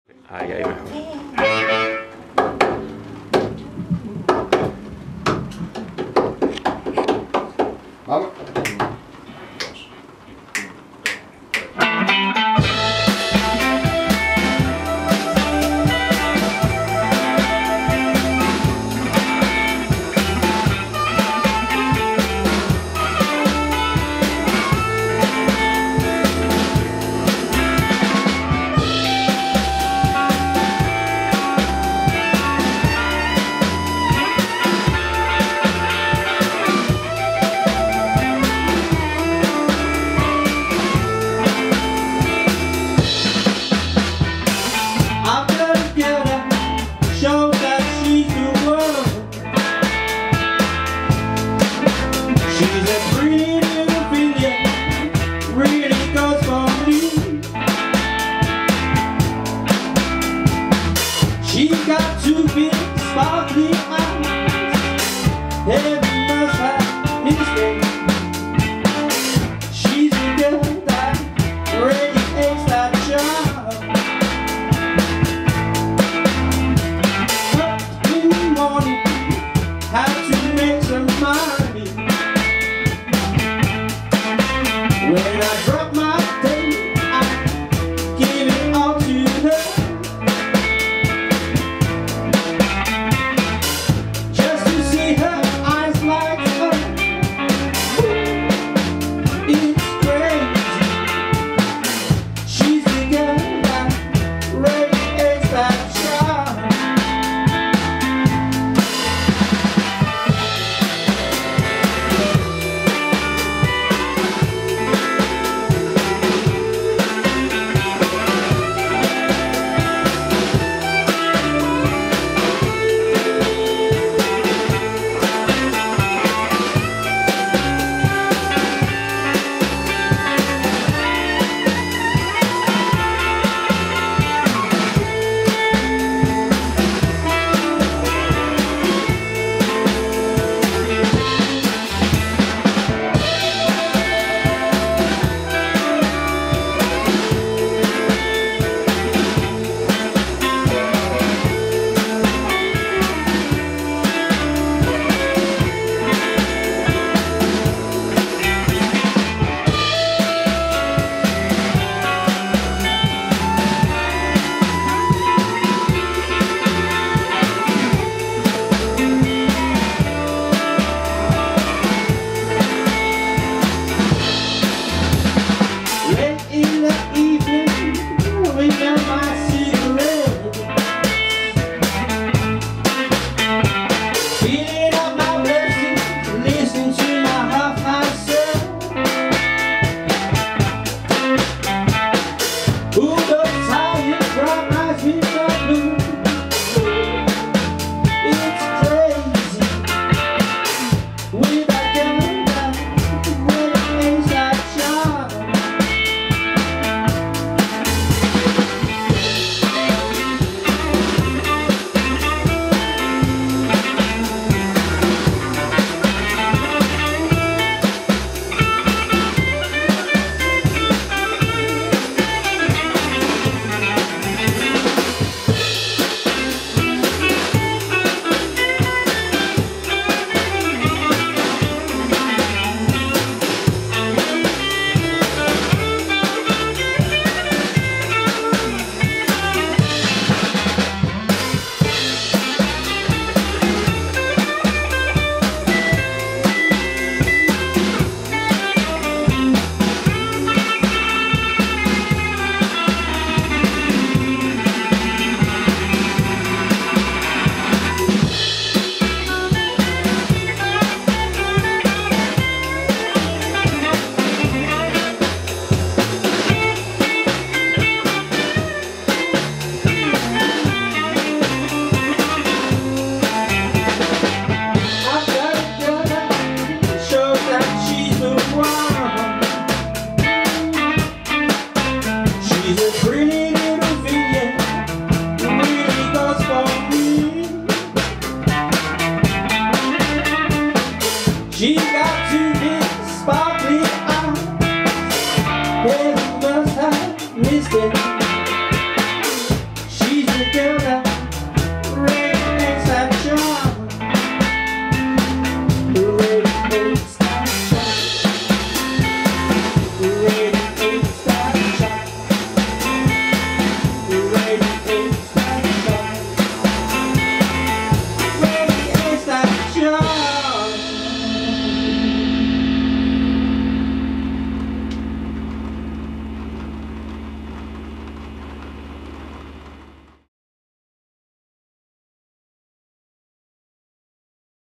BLUE STOMPERS - Grabación en Estudios Tarántula
Los Blue Stompers visitaron nuestros estudios para grabar varios temas en directo.